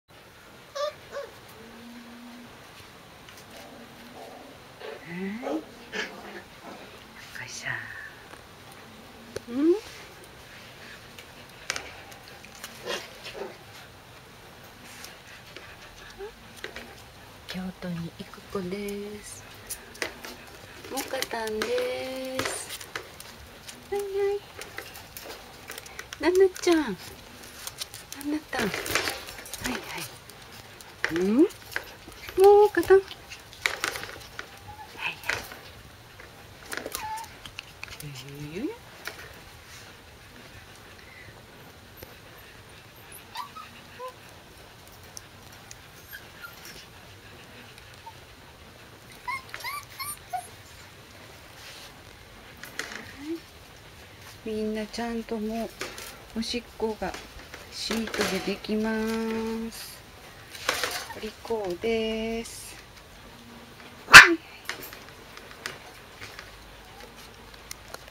コーギー仔犬 ２０１３年８月１２日
juniorとルーチェの子供たち♪お盆明けにチックン注射に行きます♪